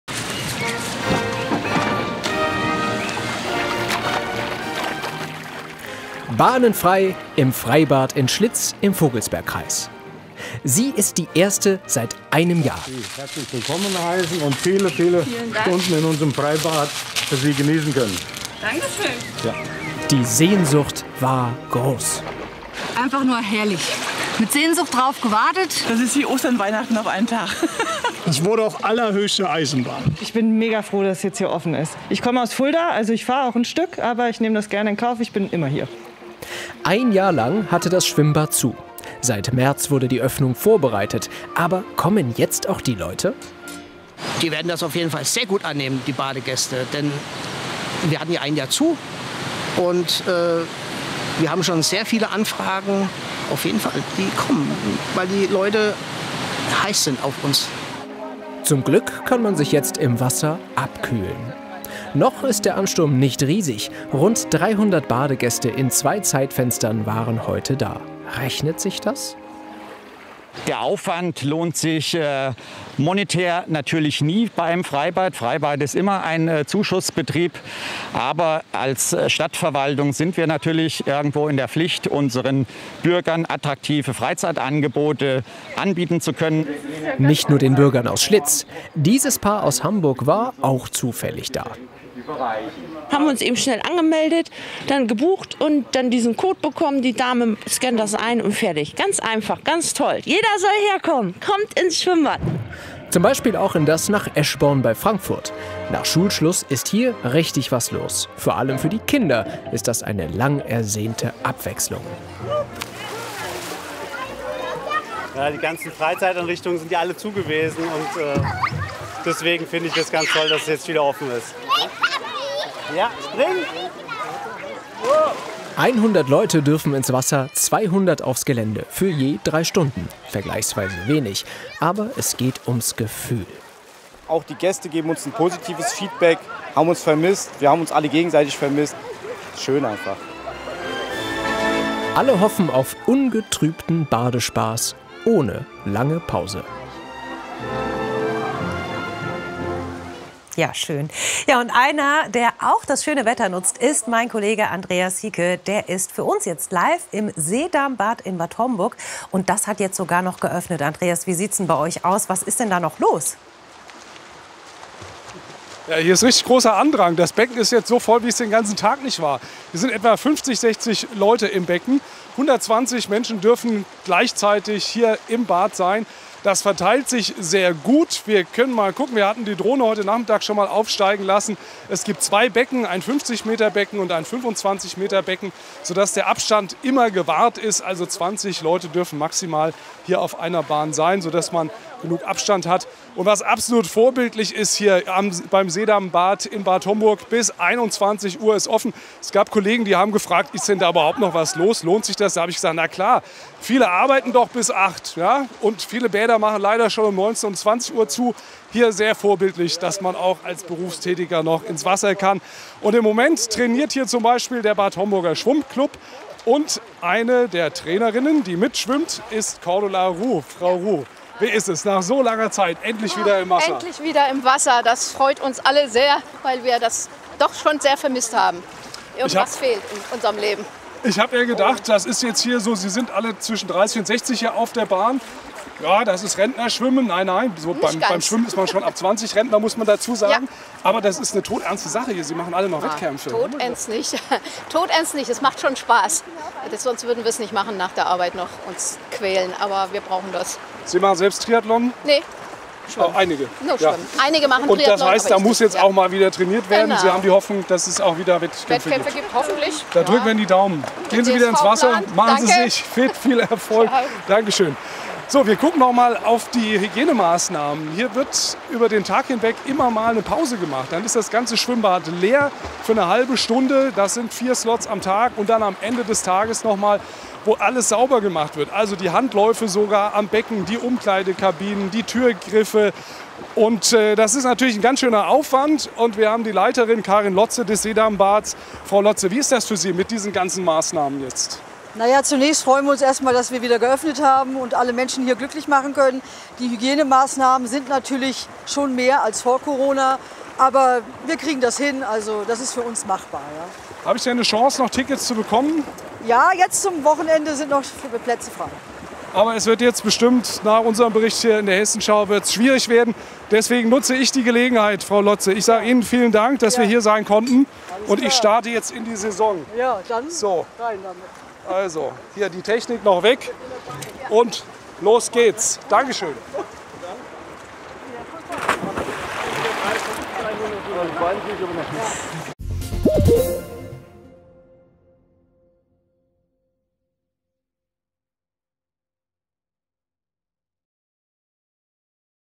Hessens Freibad-Saison geht los | hessenschau
Pünktlich zum meteorologischen Sommeranfang öffnen in Hessen viele Freibäder. Das Wetter und die Inzidenzen spielen mit, denn viele Kreise befinden sich aktuell in Stufe 2 der Corona-Verordnung des Landes.

Aber auch die Bäder in den Kreisen, die noch nicht in die Stufe 2 fallen, sind größtenteils startklar: Die Hygienekonzepte stehen, die Becken sind gefüllt und die Onlinebuchungsmöglichkeiten eingerichtet.